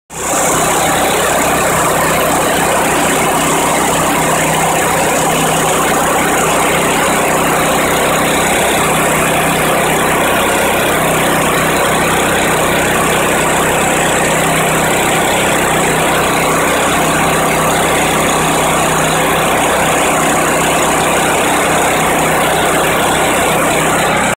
Venturi Effect - LOUD
Fuel being drawn into the carburetor as a result of the Venturi effect.